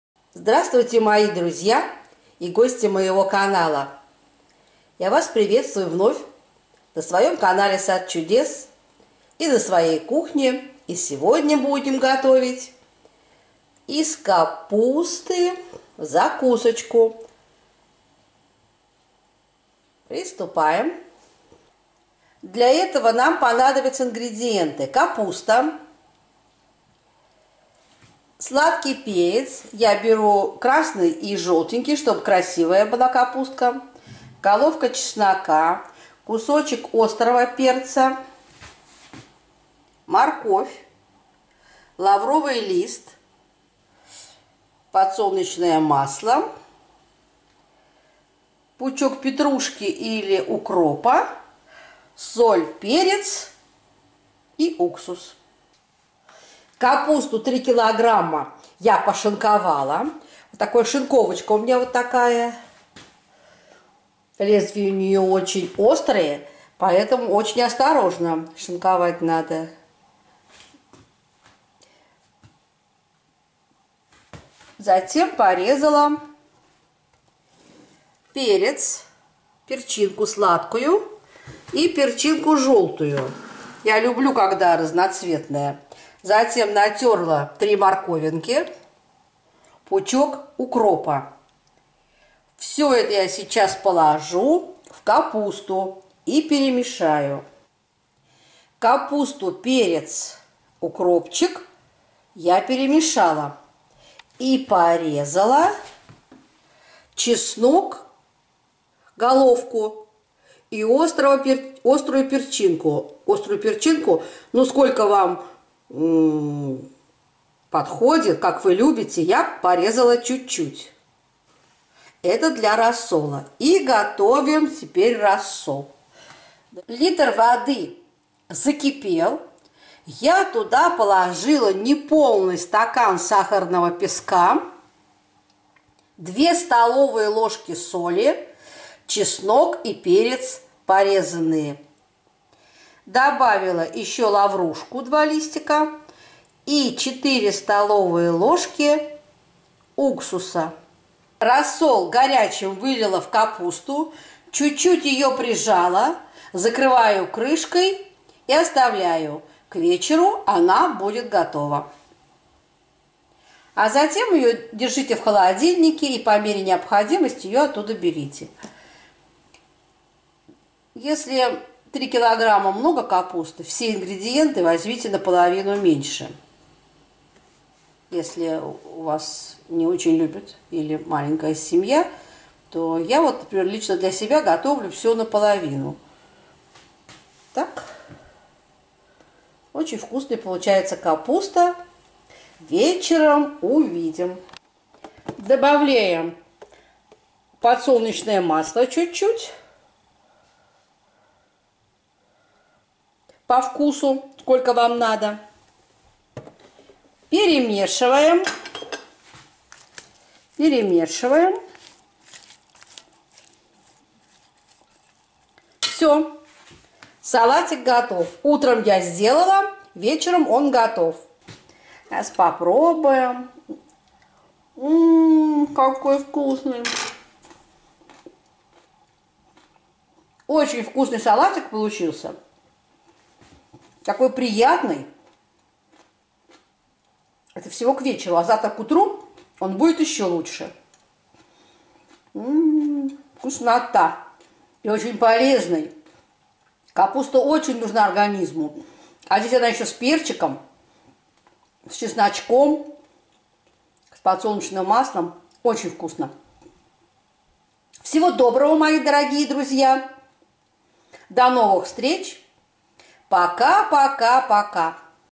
САЛАТ ИЗ КАПУСТЫ и др. овощей.
РЕЦЕПТ: 3кг.--капусты, 3ср.---моркови, 1головка чеснока,пучок зелени, 2 шт.--сладкий перец, острый перец--по вкусу, 4ст.ложки--уксуса 9%, подсолн. масло по вкусу, 1 л воды, неполный стакан --сахара, 2 ст.л.--соли, 2 лаврового листа.